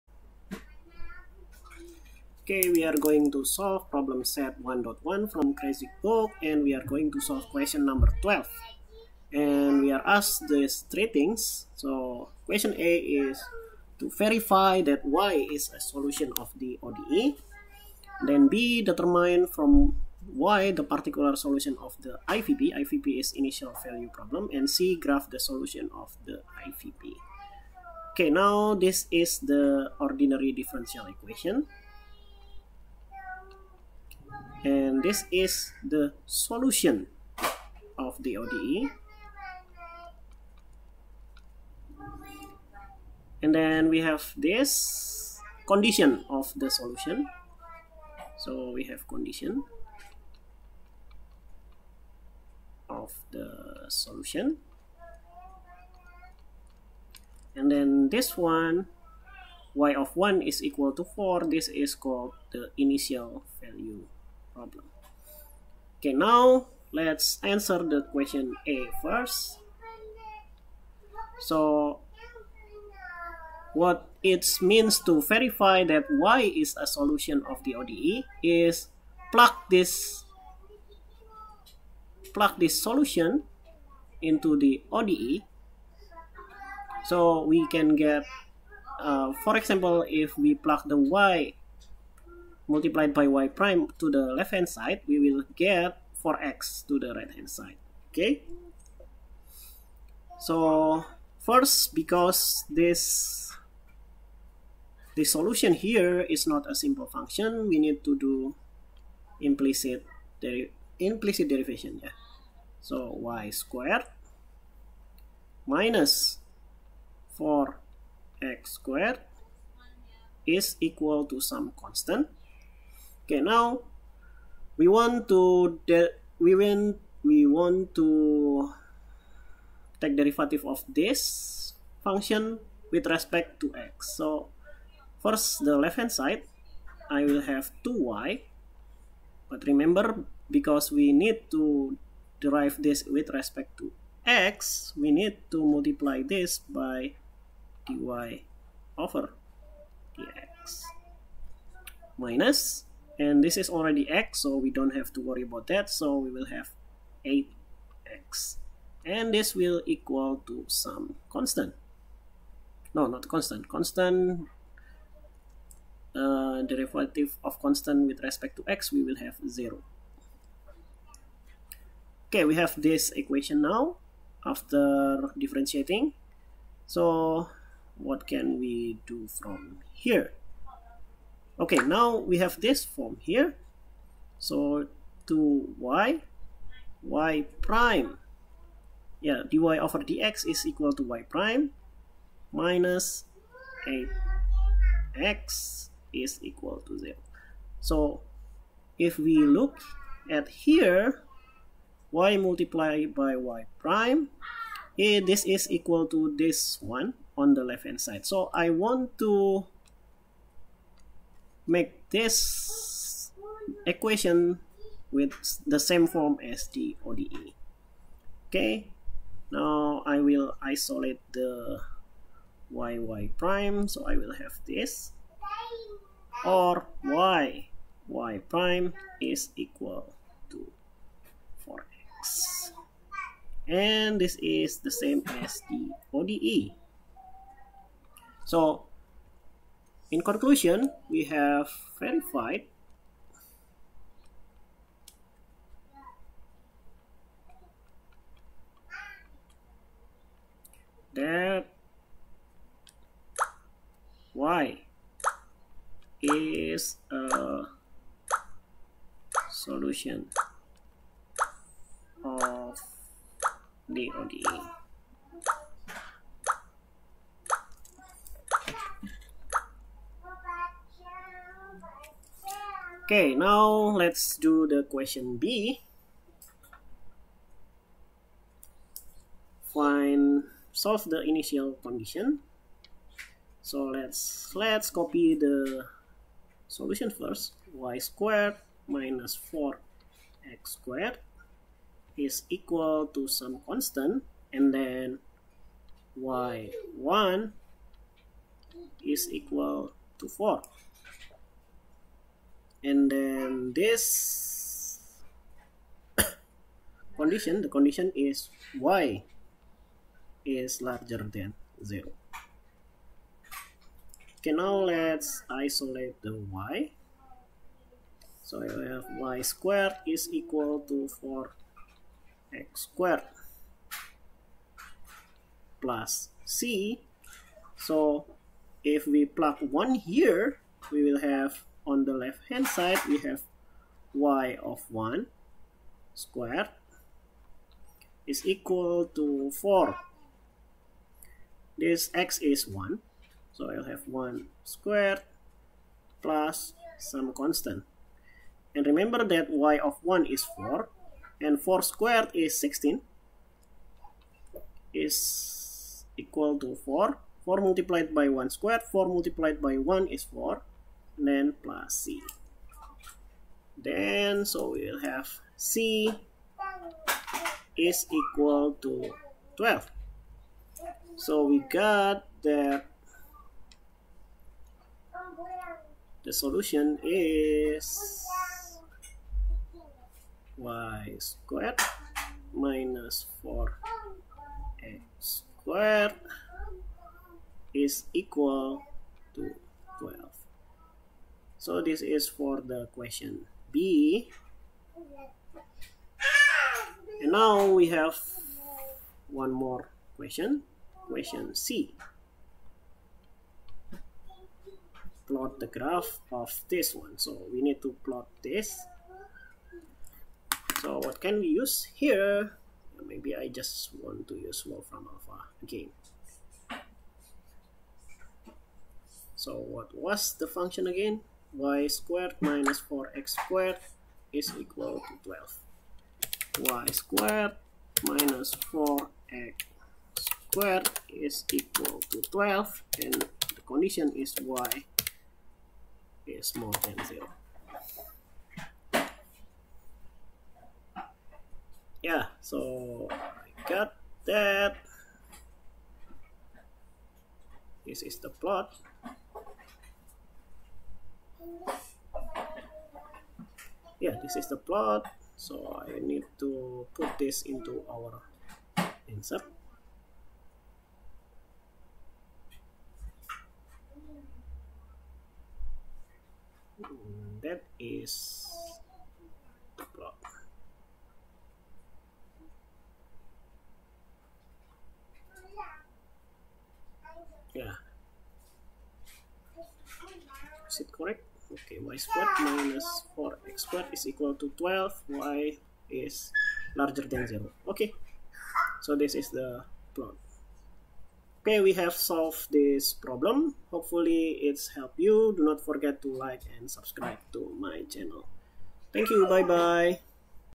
Kreyszig - Advanced Engineering Mathematics 10th Ed - Problem 1.1 Question 12
(a) Verify that y is a solution of the ODE. (b) Determine from y the particular solution of the IVP. (c) Graph the solution of the IVP.